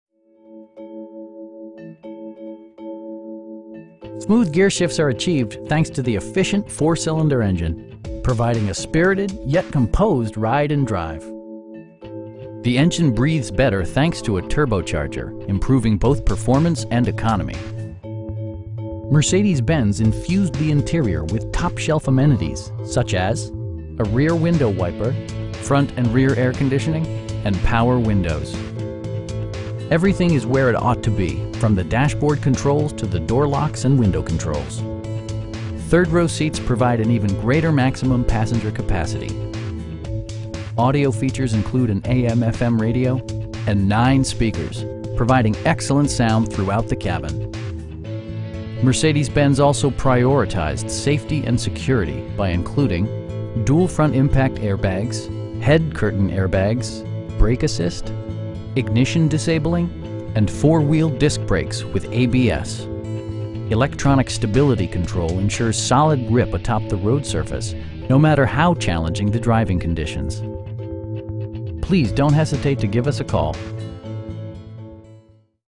2016 Mercedes-Benz Metris RWD in Lakeland, FL 33805
Fields Motorcars
4141 N. Florida Ave.

Smooth gearshifts are achieved thanks to the efficient 4 cylinder engine, providing a spirited, yet composed ride and drive. The engine breathes better thanks to a turbocharger, improving both performance and economy. Mercedes-Benz infused the interior with top shelf amenities, such as: delay-off headlights, a rear window wiper, front bucket seats, front and rear air conditioning, remote keyless entry, an overhead console, and power windows. Everything is where it ought to be, from the dashboard controls to the door locks and window controls. Storage solutions are integrated throughout the interior, demonstrating thoughtful attention to detail. Third row seats provide an even greater maximum passenger capacity. Audio features include an AM/FM radio, and 9 speakers, providing excellent sound throughout the cabin. Mercedes-Benz also prioritized safety and security by including: dual front impact airbags, head curtain airbags, traction control, brake assist, ignition disabling, and 4 wheel disc brakes with ABS. Electronic stability control ensures solid grip atop the road surface, no matter how challenging the driving conditions. You will have a pleasant shopping experience that is fun, informative, and never high pressured. Please don't hesitate to give us a call.